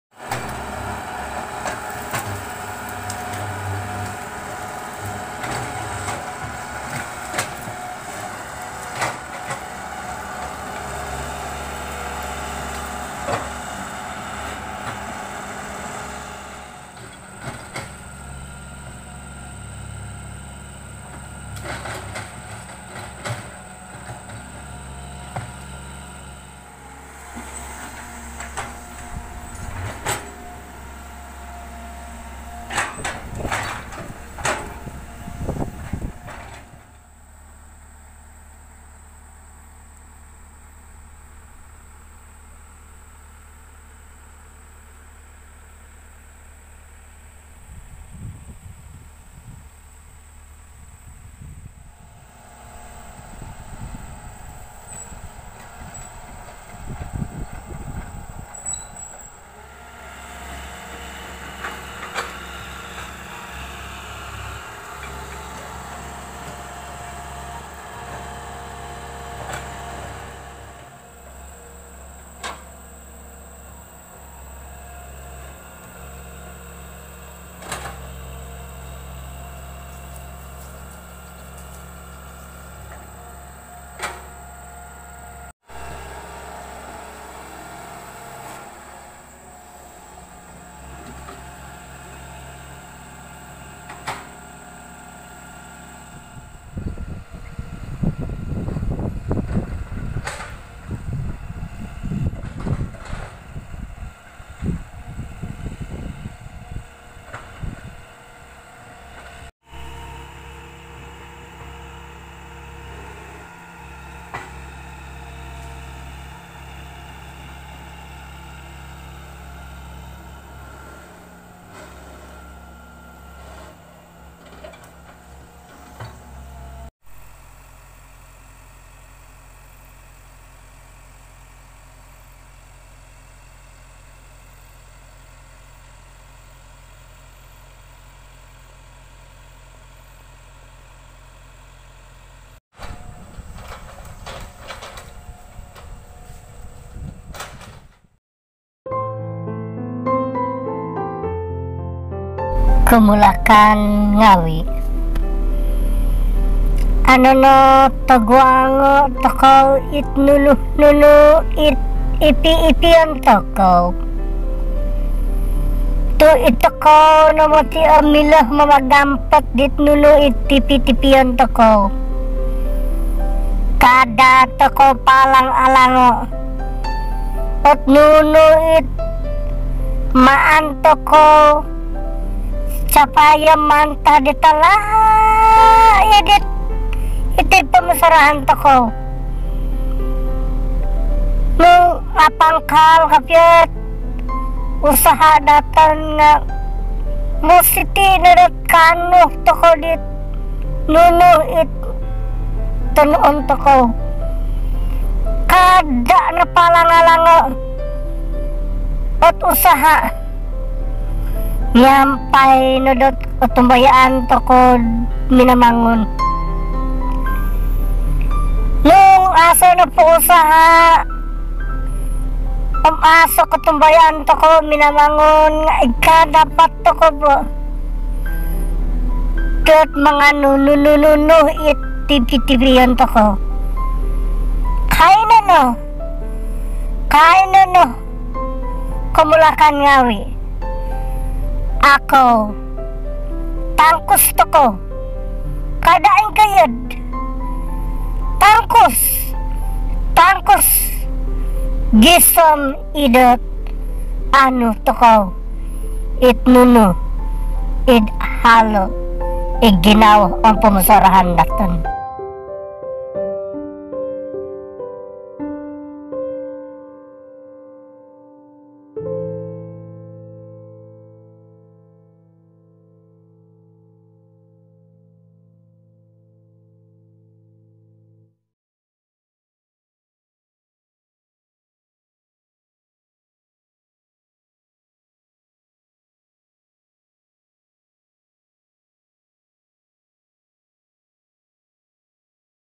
Vlog 1 || D~Yama View Garden ??? || Satu-satunya 'Resort' Ala Kampung Bertaraf 3 Bintang???
Projek ini terletak di Kebun RISDA @ Tanah Pusaka Pomoluton. Projek ini dalam rangka Fasa Pertama. Nantikan Fasa Kedua dalam video selanjutnya.

Please visit and like our official page on Facebook & follow our Instagram;